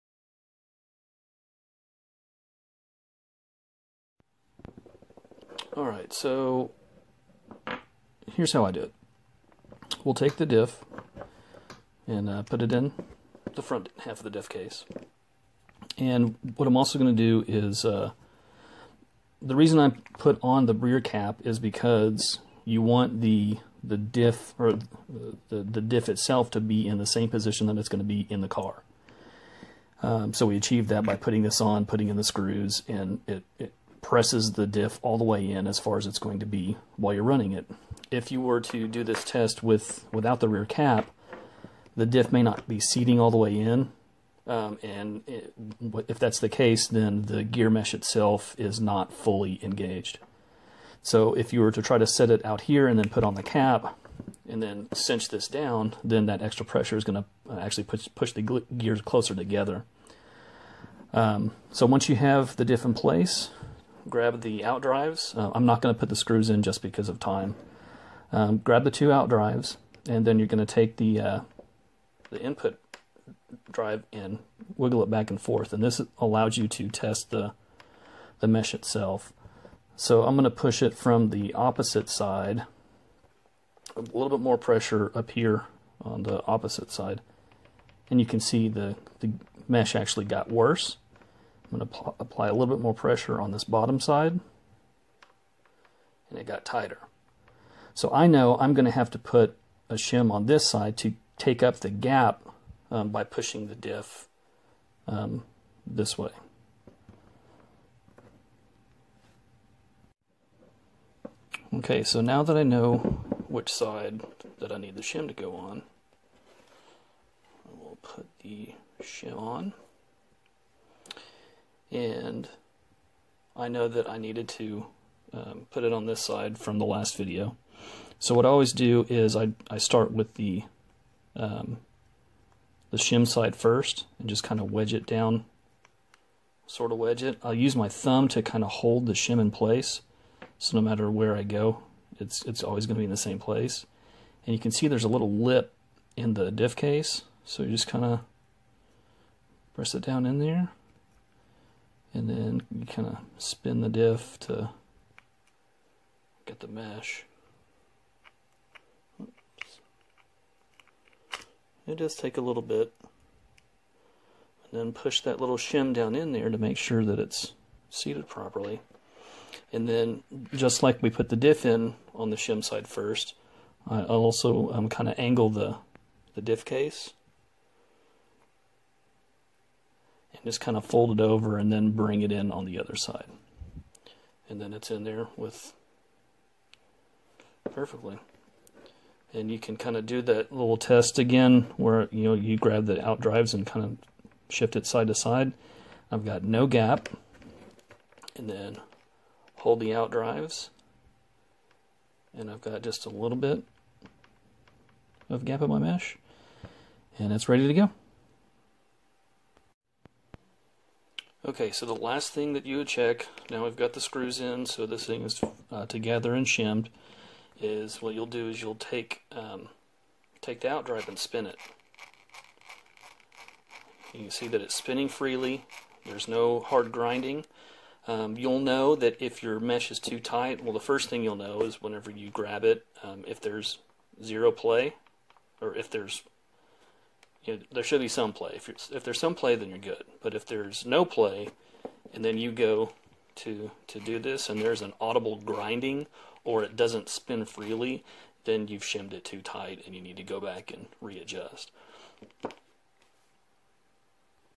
RC differential / diff shimming for dummies
Basic tutorial on what to look for when shimming a diff, this example was done on a 1/8th scale truggy diff but is applicable to smaller scale as well. Note: Every diff case can be different, some Ive had to use 3 shims, some none. Tekno RC, NT48.3, NB48 2.0, EB48 2.0, ET410 2.0, EB410 2.0